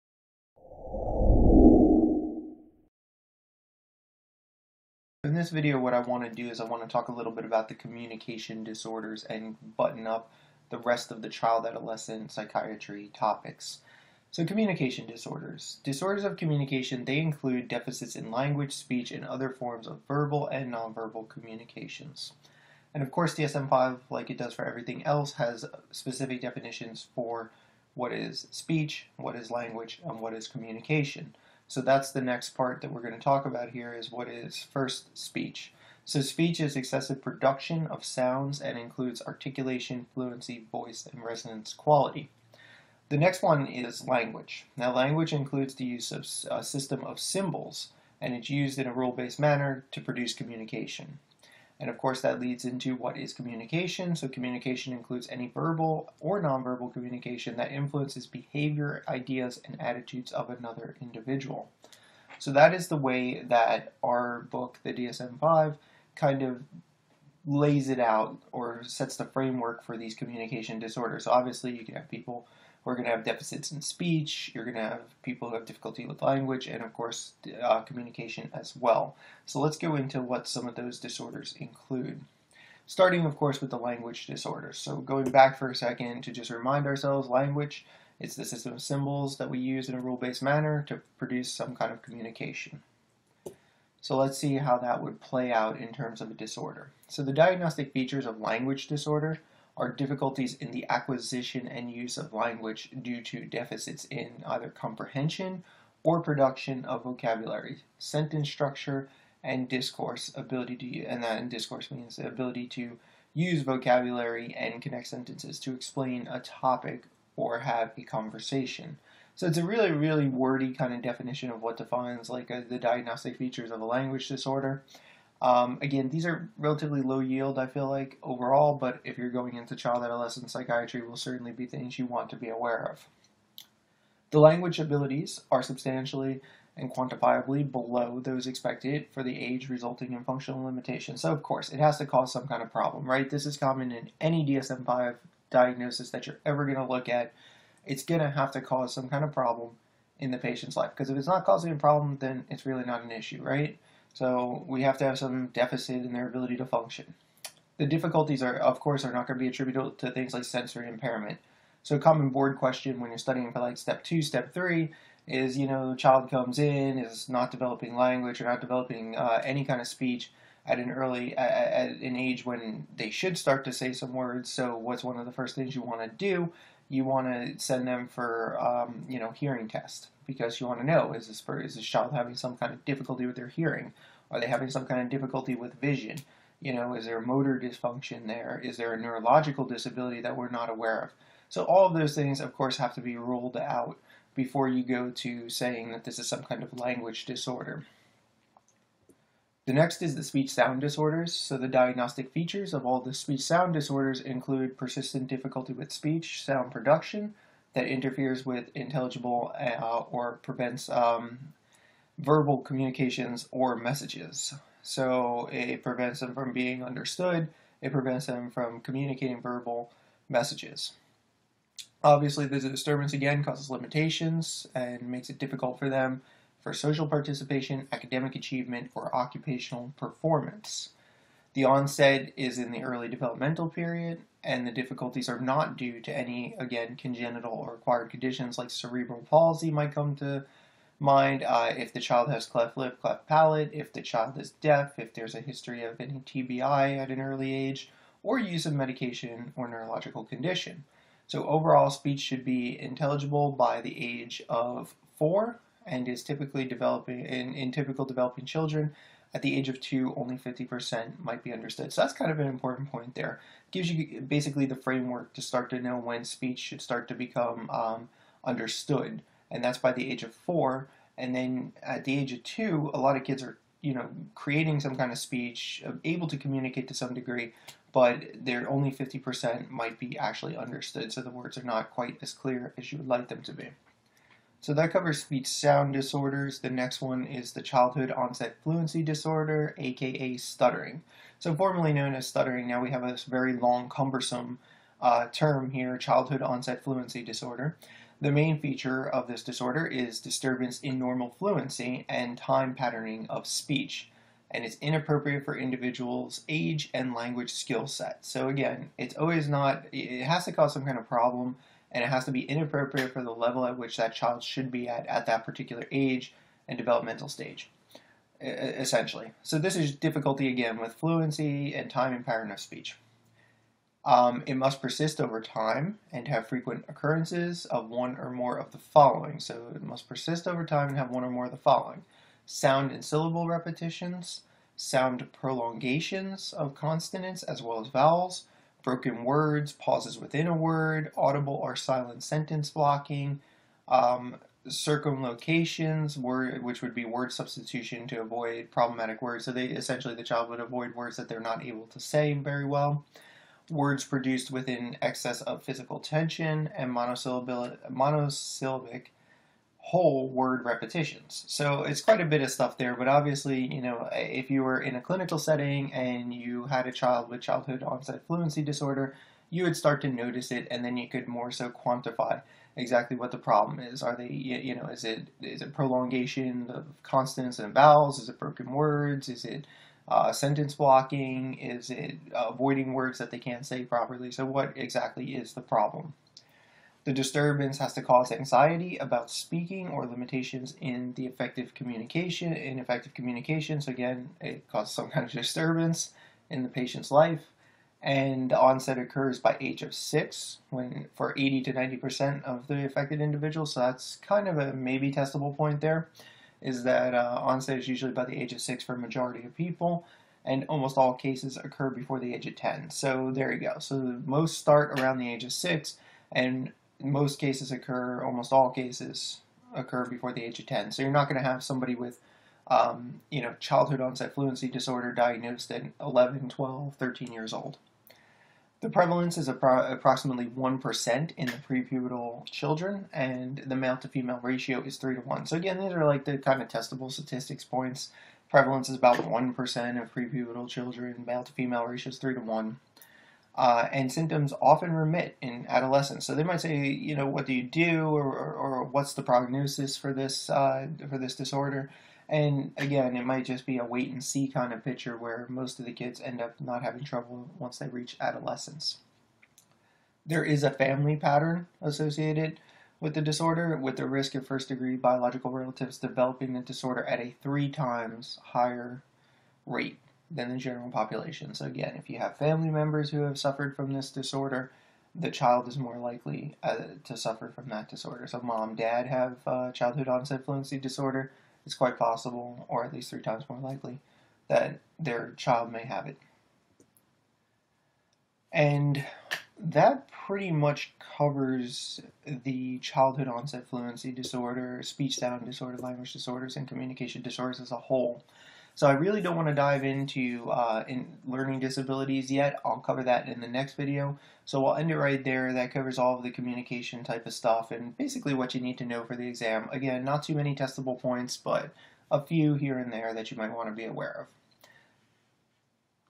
Psychiatry Review: Communication Disorders
In this video I go over the important communication disorders including the definition of speech, language, and communication. I also cover the language disorders, speech sound disorder, and childhood onset fluency disorder (stuttering).